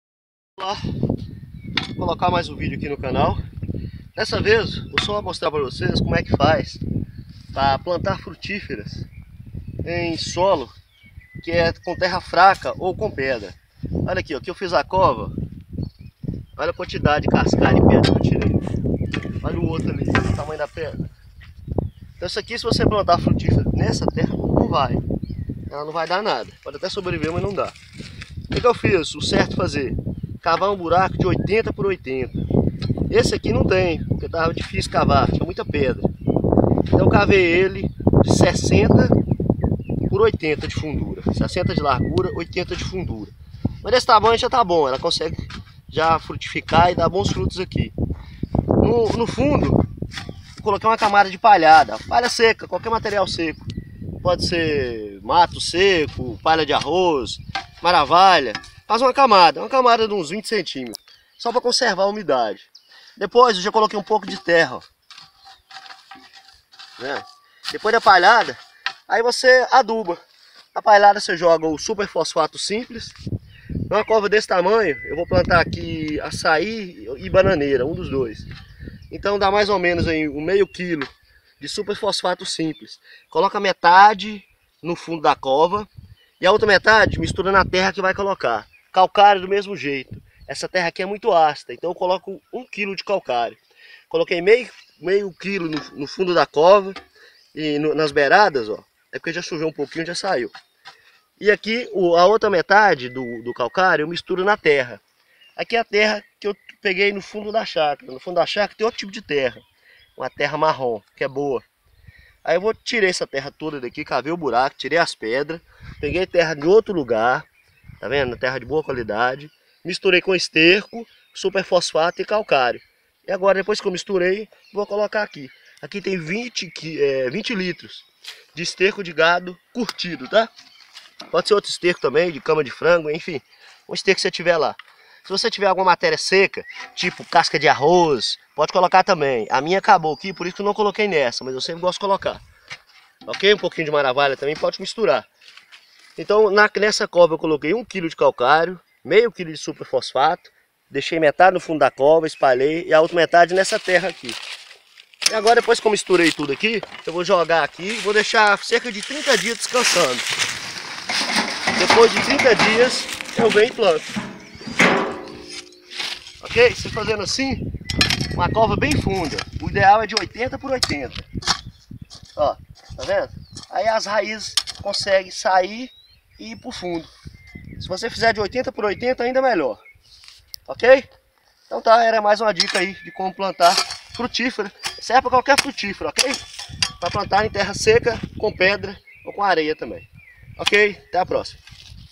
Como plantar frutíferas em solo fraco ou com pedras.
Como plantar frutíferas em solo fraco ou com pedras, mostrando:
1) o tamanho da cova: 80 x 80 cm (ou 80 profundidade x 80 largura cm);
2) adubação química: 500g de superfosfato simples ou 300g do supersimples;
3) adubação natural: 5 kg de esterco de boi curtido;
4) palhada seca no fundo da cova.
5) calagem: 1 kg de calcário quando a terra é muito ácida ou a meio quilo quando é pouco ácida ou não foi medido o ph.